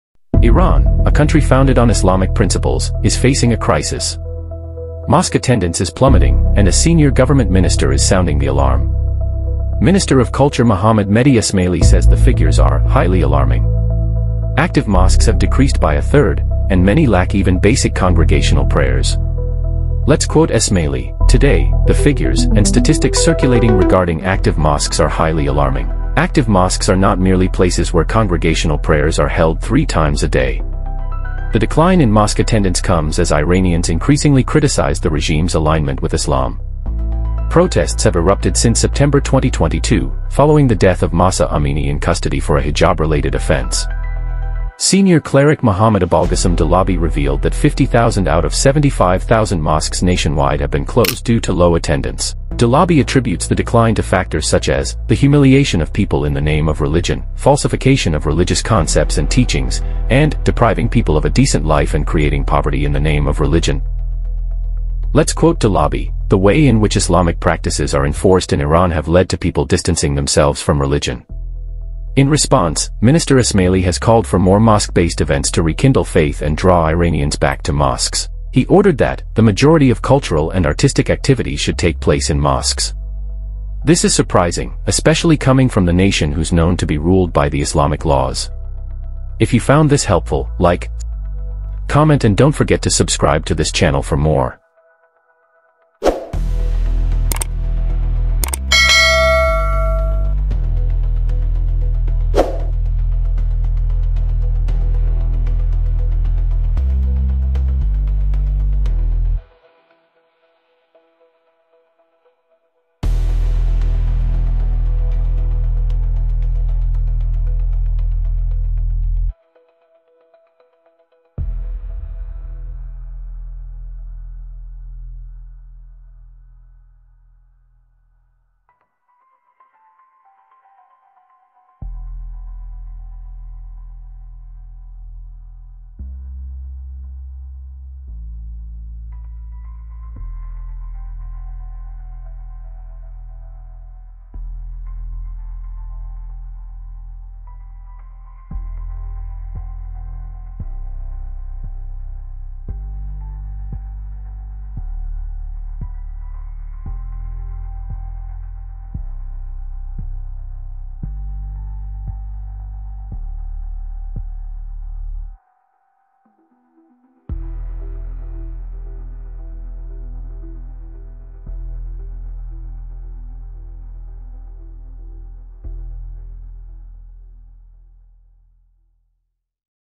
Mosques Closing Down Fast in Iran And Christianity Growing Also.
Iran's Mosque Attendance Crisis: A Senior Minister Sounds the Alarm.

Iran's mosque attendance has plummeted, with a third of active mosques closing.

 Minister of Culture Mohammad Mehdi Esmaili calls the figures 'highly alarming'.

What's behind the decline?

Protests, regime criticism, and a disconnect from Islamic principles. Watch to learn more.